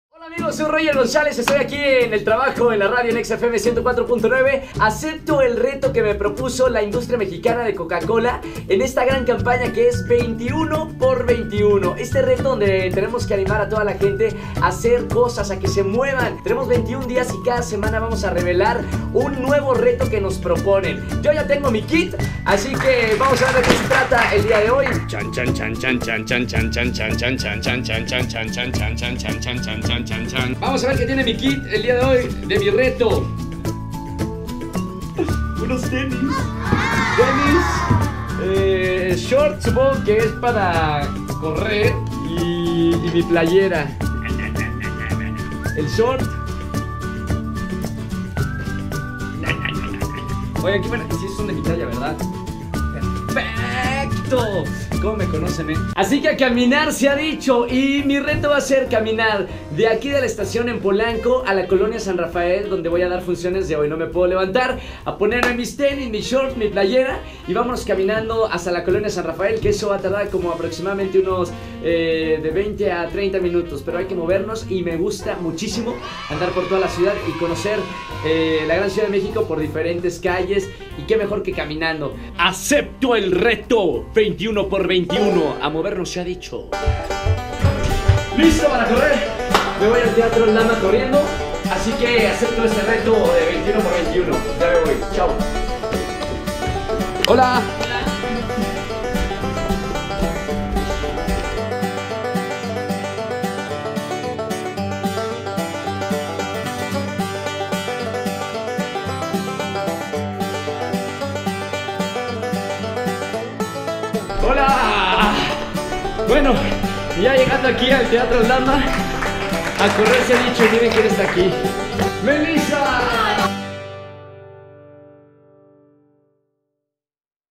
Reto21x21 Muévete verás que es divertido y te hará sonreír
Roger es parte de la gran campaña de la industria mexicana de Coca Cola en donde cada semana tendrá que cumplir un reto